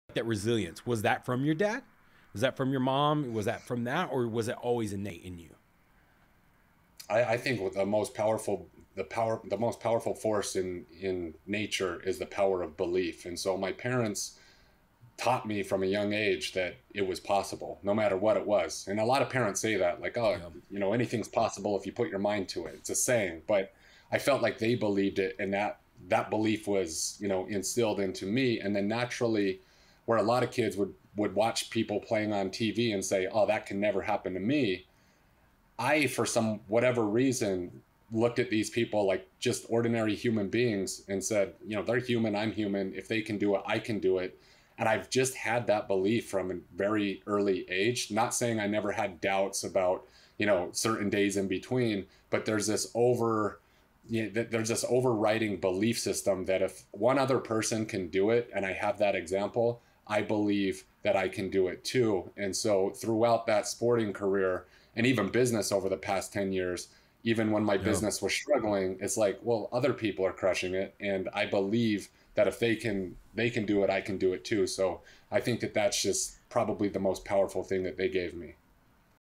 The Power Of Belief Mindset
Our beliefs have the power to drive us to success. Combine that with the right mindset and you too can make it big in sales. Check out the full podcast episode on YouTube

Do you have what it takes to become a Full Stack Sales Pro? Take our FREE quiz to find out